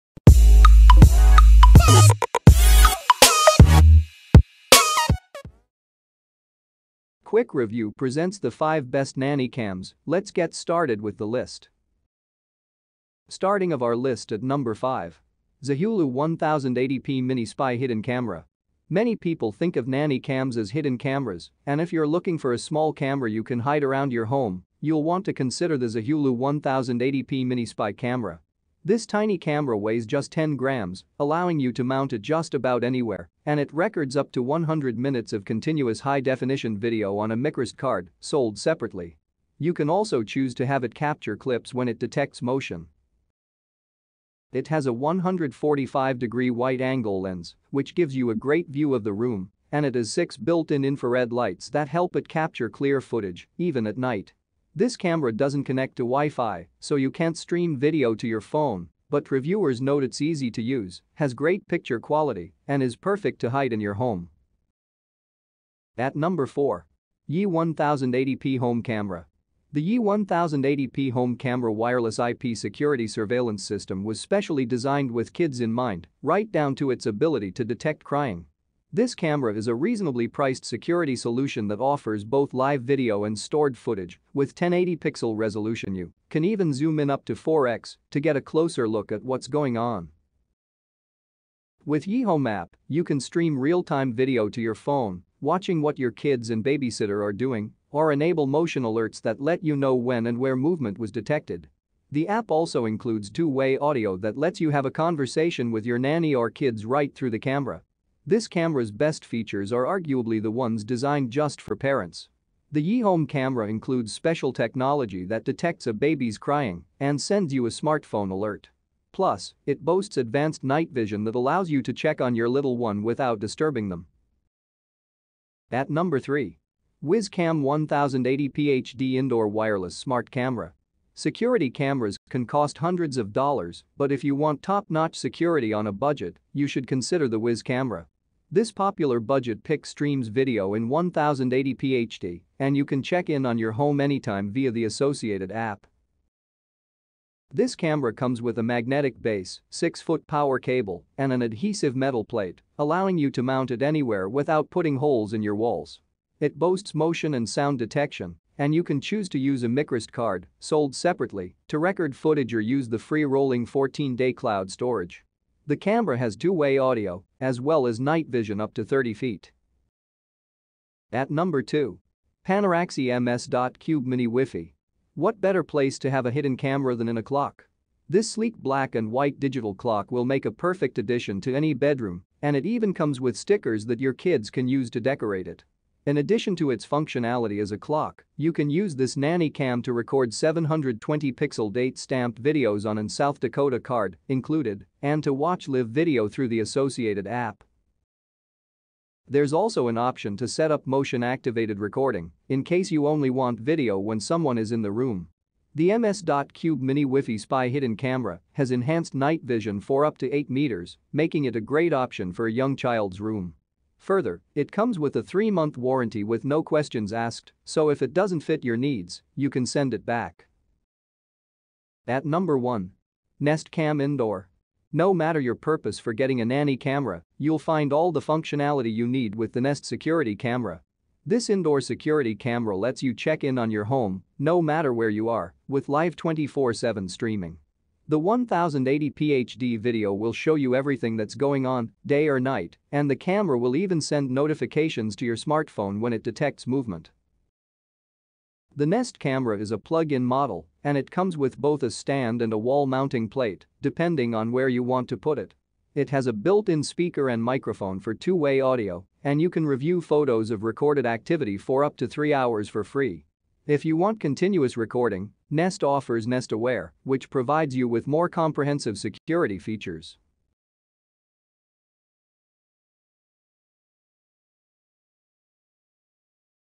The 05 Best Nanny Cams of 2020
Links of Best Nanny Cams of 2020
👉 5. Zohulu 1080P Mini Spy Hidden Camera
👉 4. YI 1080p Home Camera
👉 3. Wyze Cam 1080p HD Indoor Wireless Smart Camera
👉 2.  PANORAXY Ms.Cube Mini WiFi
👉 1. Nest Cam Indoor